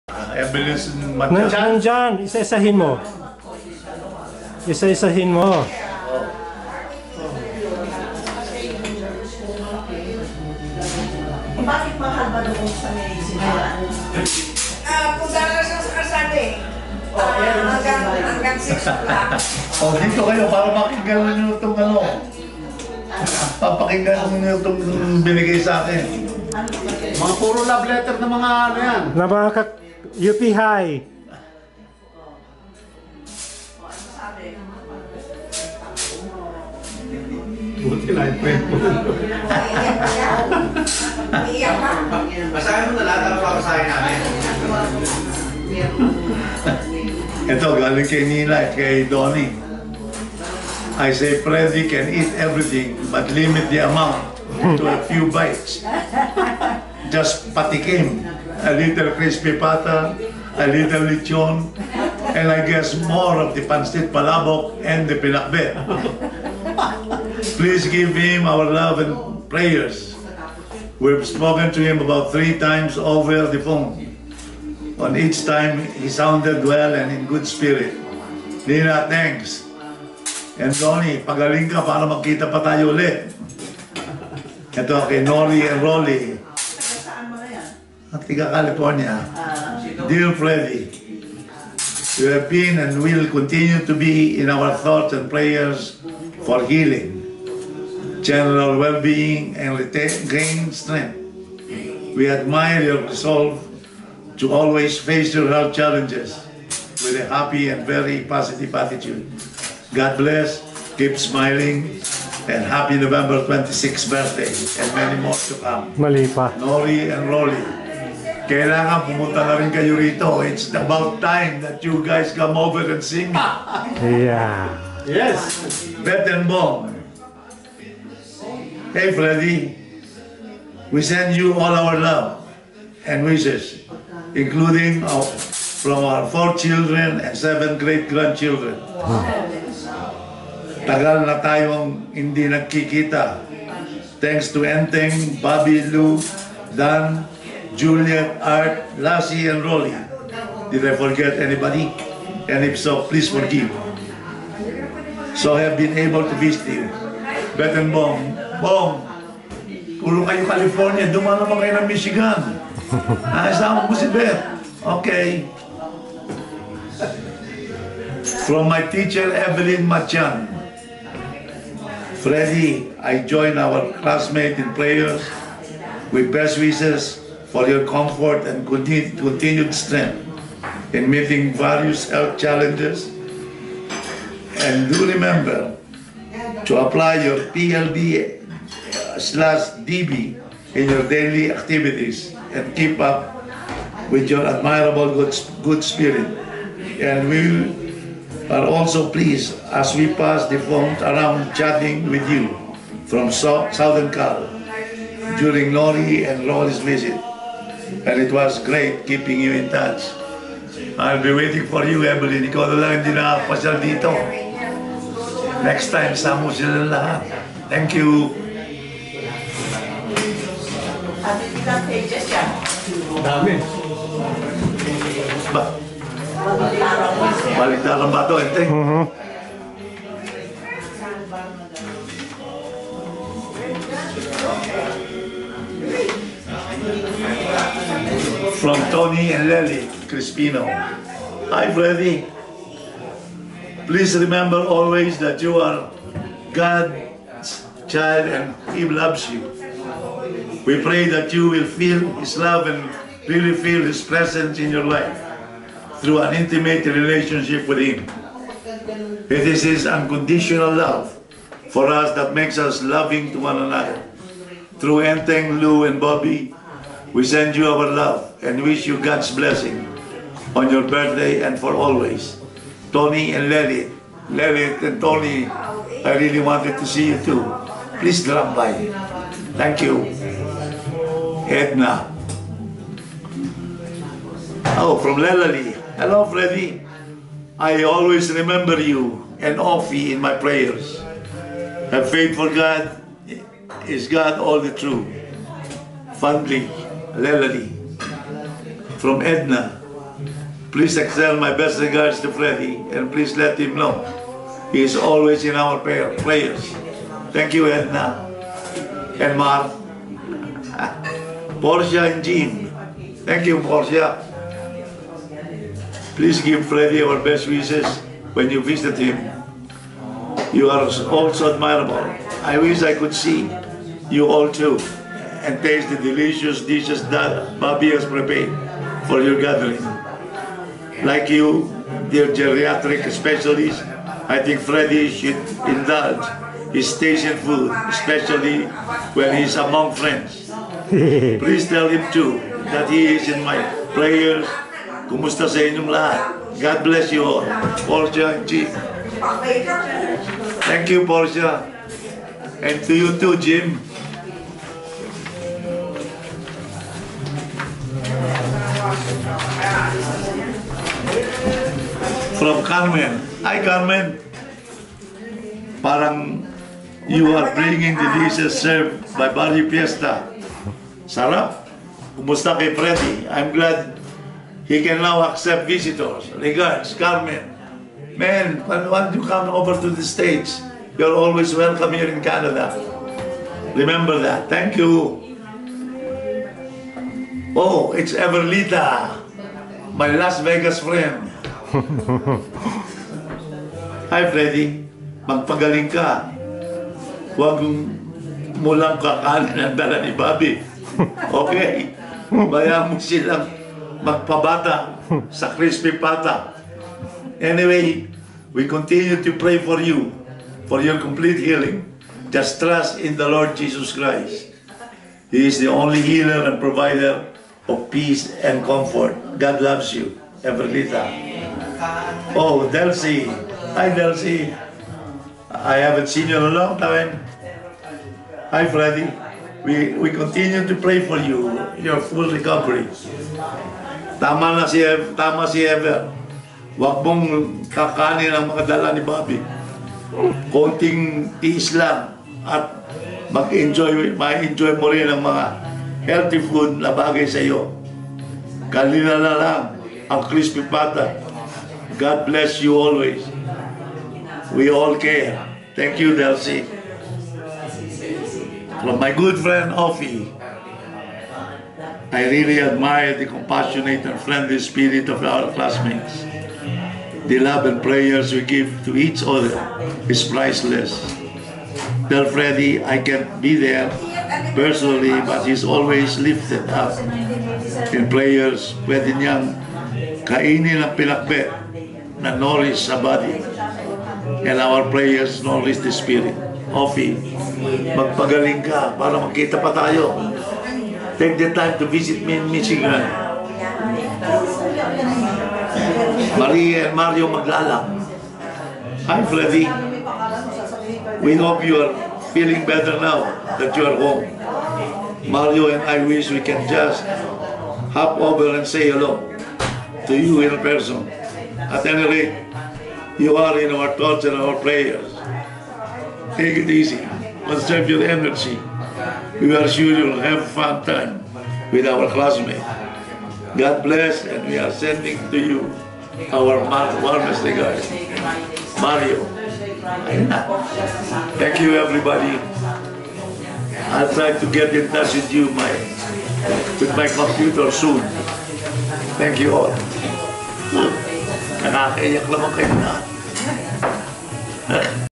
FreddieNov2014
Freddie reading classmates' greetings